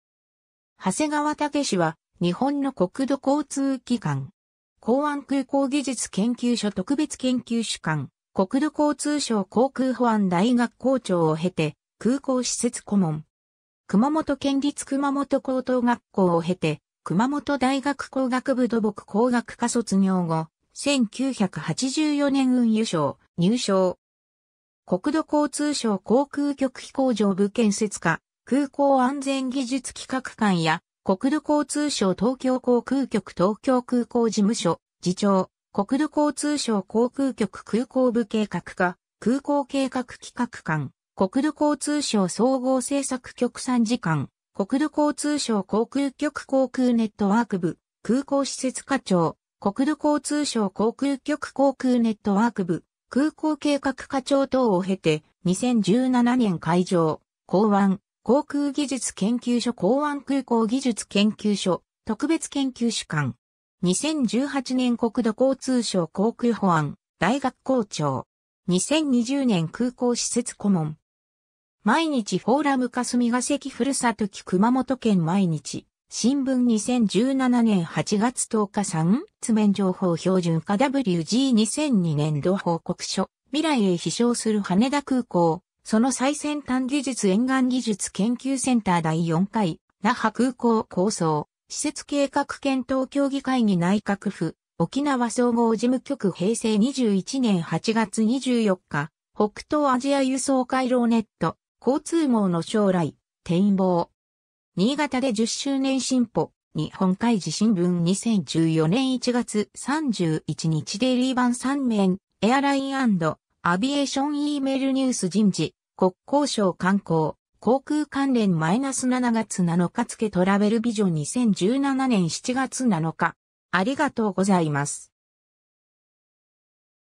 長谷川武 (国土交通技官)
長谷川 武（はせがわ たけし）は、日本の国土交通技官。
港湾空港技術研究所特別研究主幹、国土交通省航空保安大学校長を経て、空港施設顧問。
 熊本県立熊本高等学校を経て、熊本大学工学部土木工学科卒業後、1984年運輸省入省。
国土交通省航空局飛行場部建設課空港安全技術企画官や、国土交通省東京航空局東京空港事務所次長、国土交通省航空局空港部計画課空港計画企画官、国土交通省総合政策局参事官、国土交通省航空局航空ネットワーク部空港施設課長、国土交通省航空局航空ネットワーク部空港計画課長等を経て、2017年海上・港湾・航空技術研究所港湾空港技術研究所特別研究主幹。
2018年国土交通省航空保安大学校長。
2020年空港施設顧問。
 ^ 「毎日フォーラム・霞が関ふるさと記熊本県」毎日新聞2017年8月10日 ^ Ⅲ．図面情報標準化ＷＧ ２００２年度報告書 ^ 未来へ飛翔する羽田空港、その最先端技術 - 沿岸技術研究センター ^ 第４回那覇空港構想・施設計画検討協議会 議内閣府沖縄総合事務局平成２１年８月２４日 ^ 北東アジア輸送回廊ネット/交通網の将来、展望。
新潟で10周年シンポ日本海事新聞2014年01月31日 デイリー版3面 ^ Airline & Aviation E-mail News ^ 人事、国交省観光・航空関連－7月7日付トラベルビジョン2017年7月7日（金 ^ 国土交通省人事産経新聞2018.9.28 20:51 ^ 国家公務員法第106条の25第１項等の規定に基づく国家公務員の再就職状況の報告（令和２年７月１日～同年９月30日分）